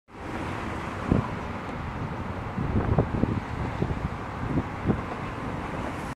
F10 M5 Turbo Supercar
Occurring at the moment of the F80 BMW M3 flyby, this BMW F10 M5 was up first. The F80 almost caught me by surprise going by. So I have two M cars, at the same time. More videos upcoming.